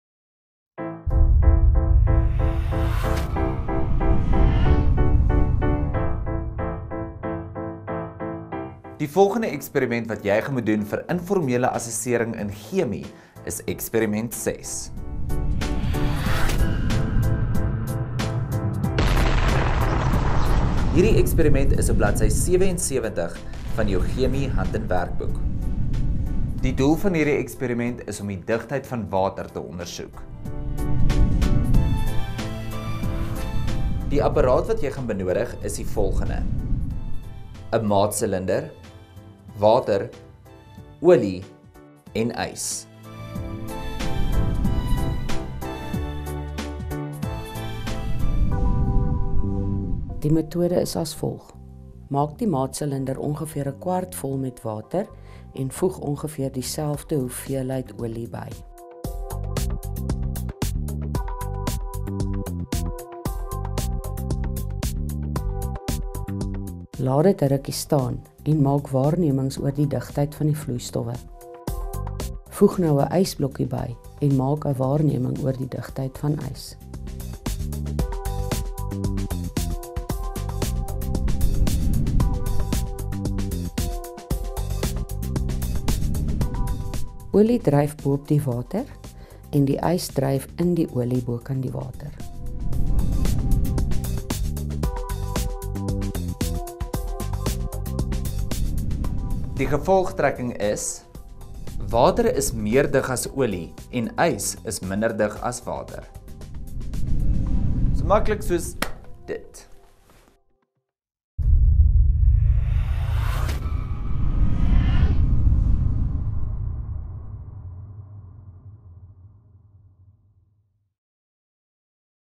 Gr.11Chemie Eksperiment 6 Afrikaans
Graad 11 Chemie informele eksperiment 6, Digtheid van Water, deel van die 24 eksperiment videos op ons USB